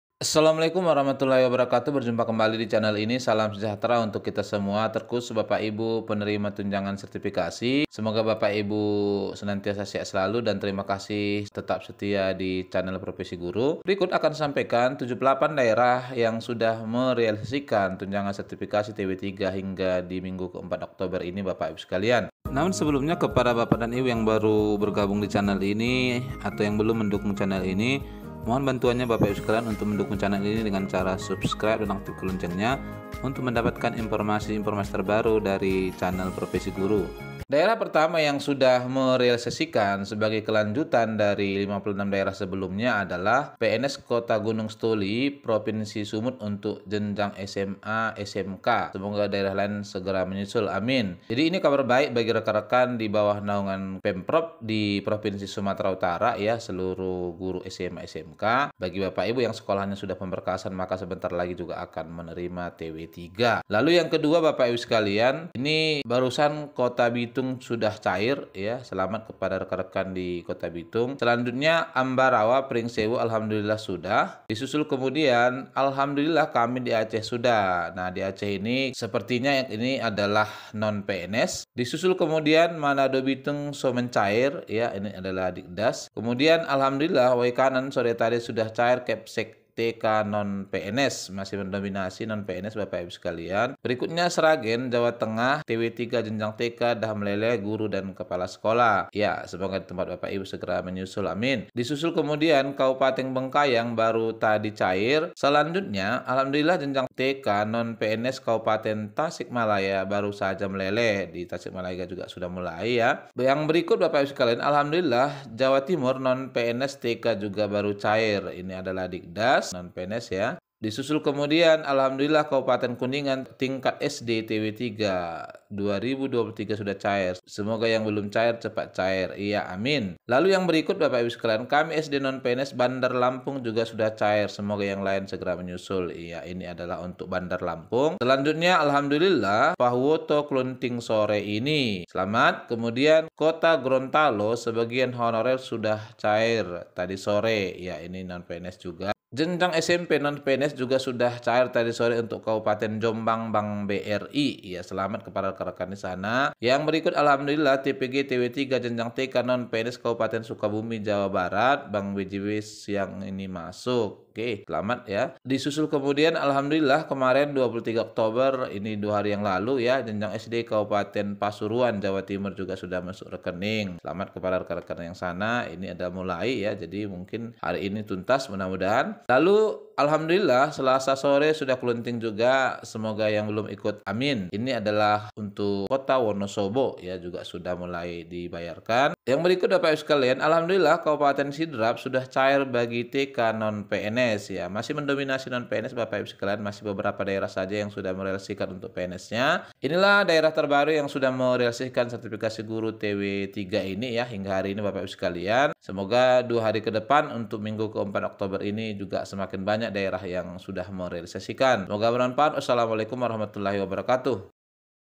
🔴 KABAR TERBARU PENCAIRAN SERTIFIKASI GURU TW 3 TAHUN 2023 MINGGU KE 4 OKTOBER
pencairansertifikasigurutw32023

Informasi terbaru terkait Daerah yng sudah mencairkan sertifikasi guru tw 3 tahun 2023. cek informasi selengkapnya